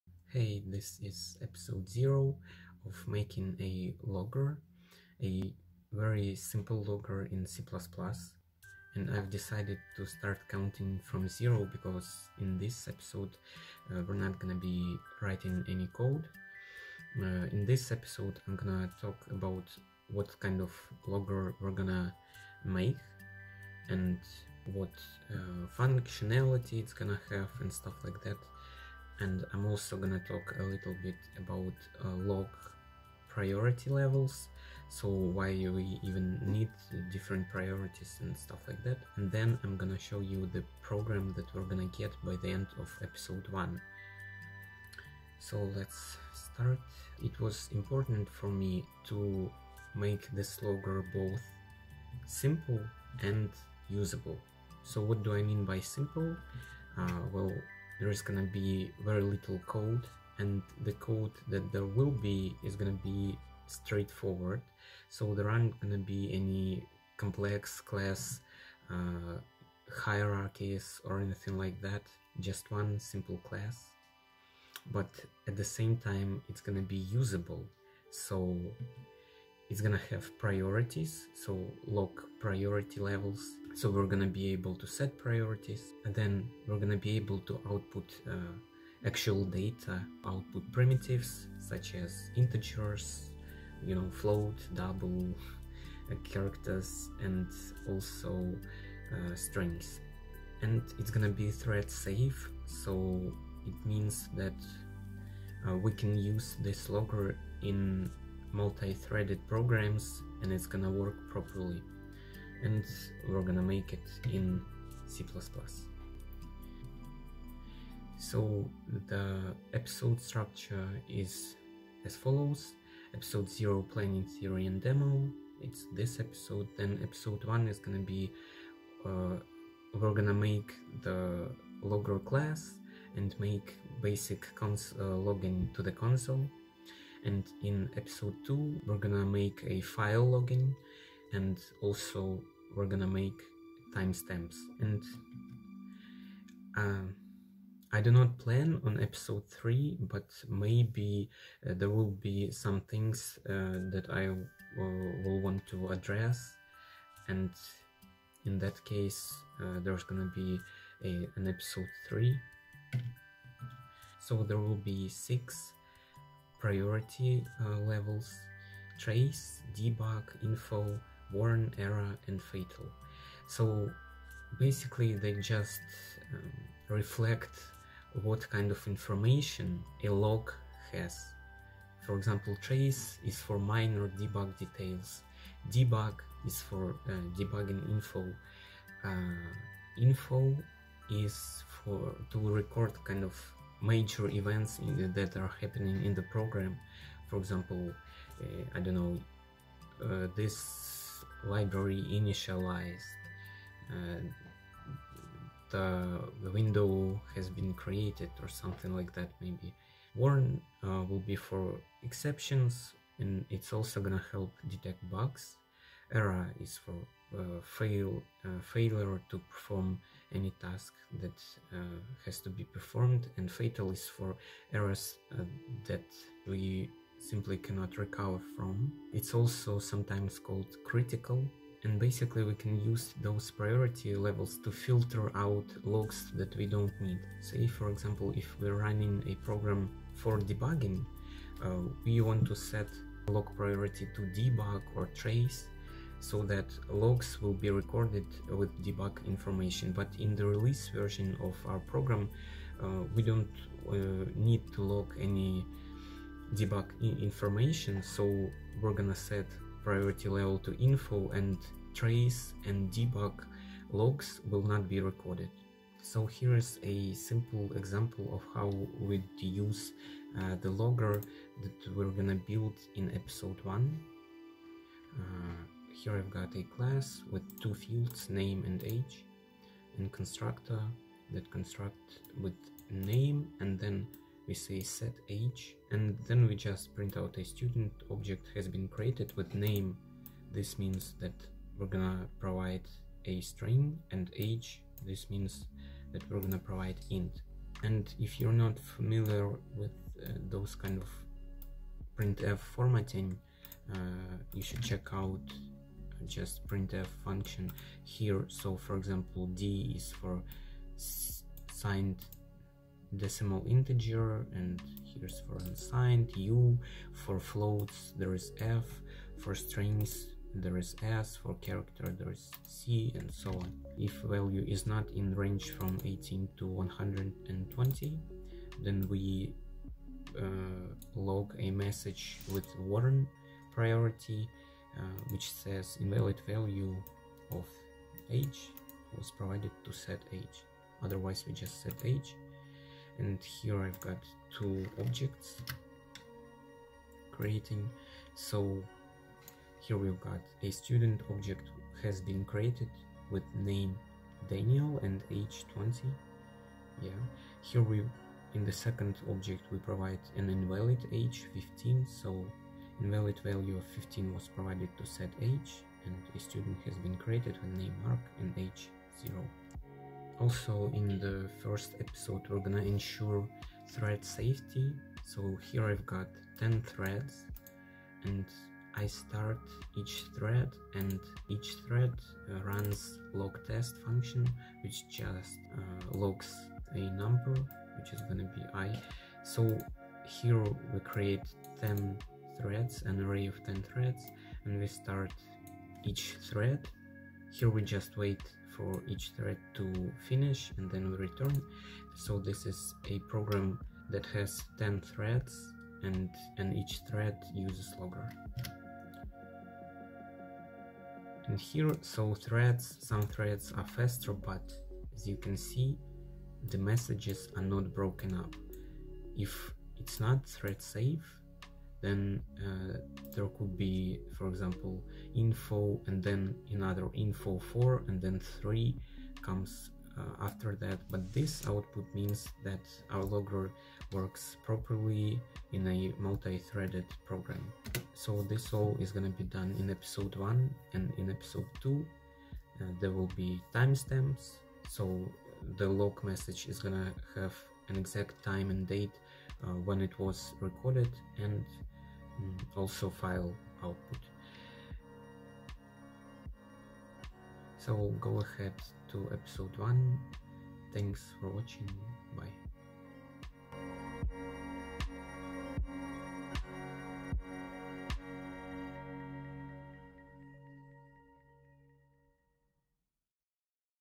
Simple Logger Ep. 0 Intro (How To Make a Logger in C++) (Beginner Project C++)
This is episode 0 of making a simple logger in C++. In this episode I talk about the logger we're building and what log priority levels are.
In episode 1 we'll be implementing console logging and ensuring thread safety.
In episode 2 we'll be implementing file logging and timestamps.